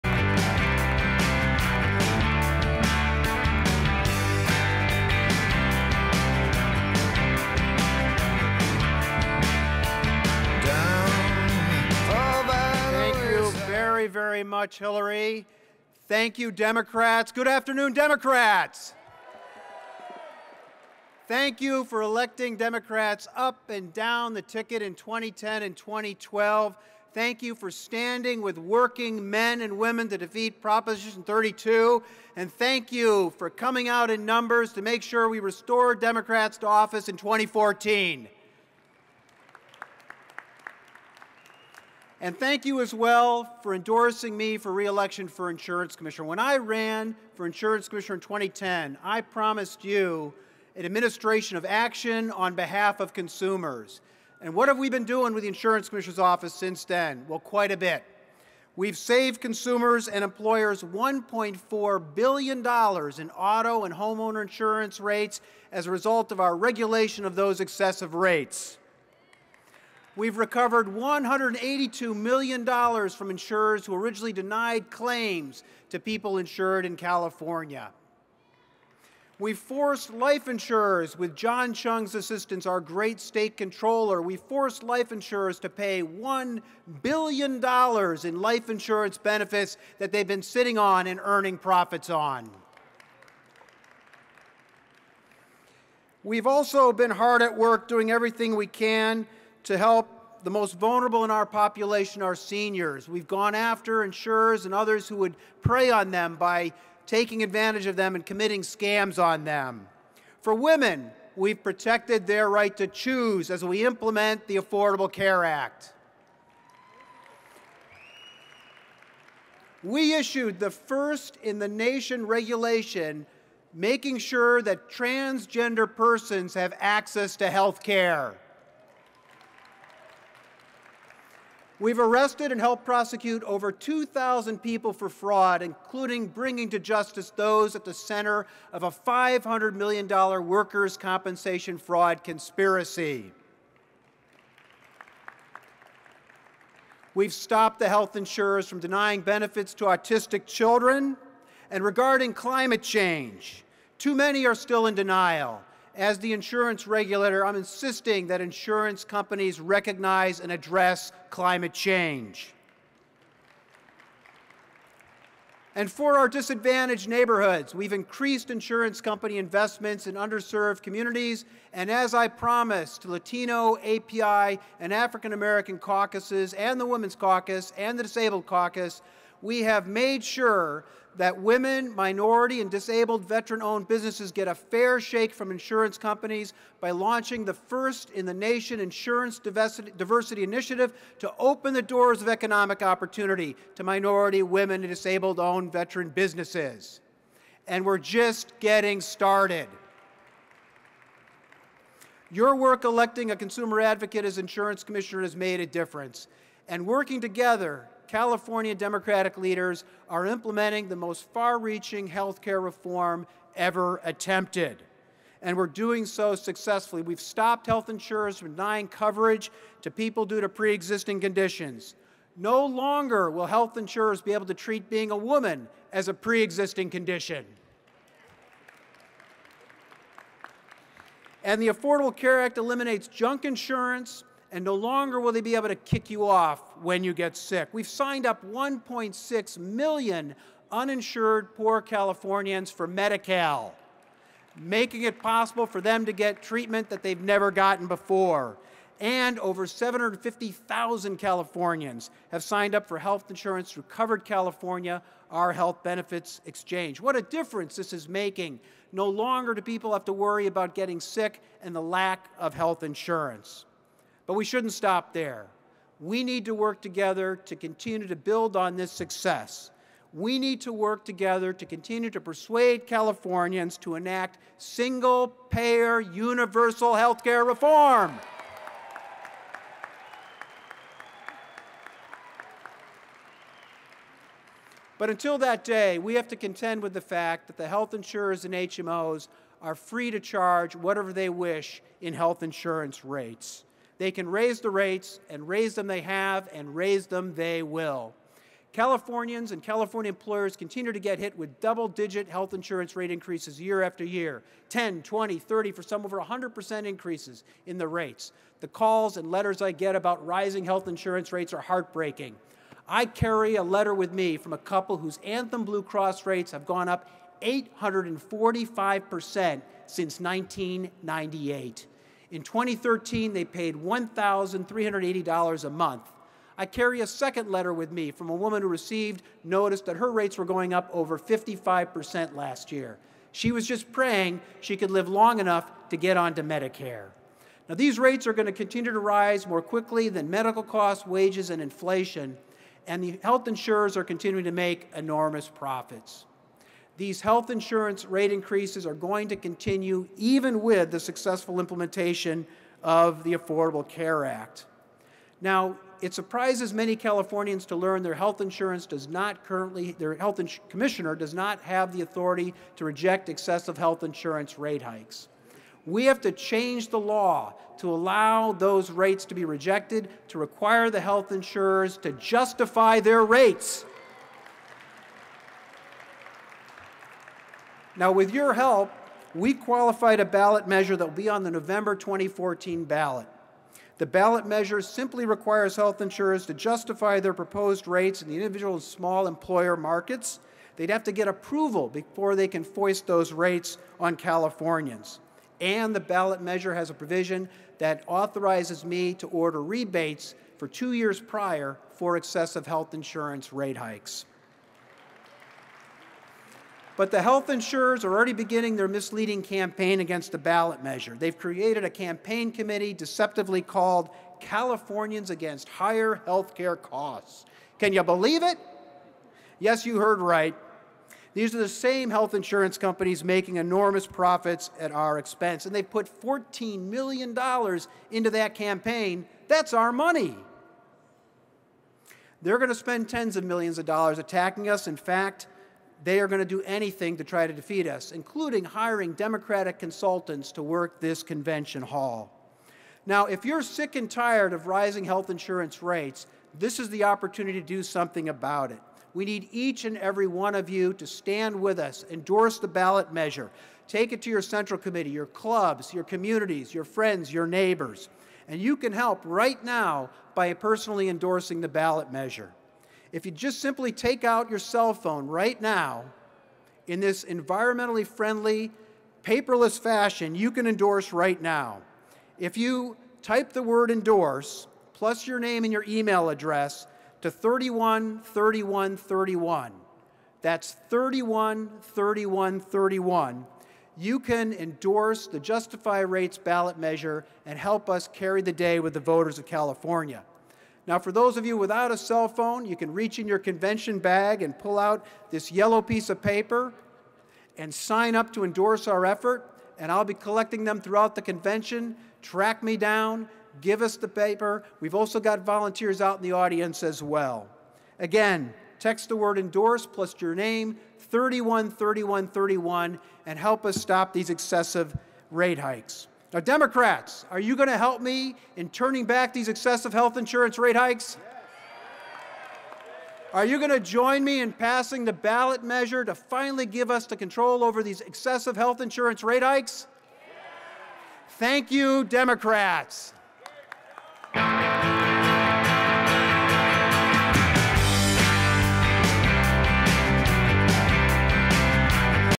CA Insurance Commissioner Dave Jones 2014 CADEM Convention
California Insurance Commissioner Dave Jones addresses delegates at the 2014 California Democrats State Convention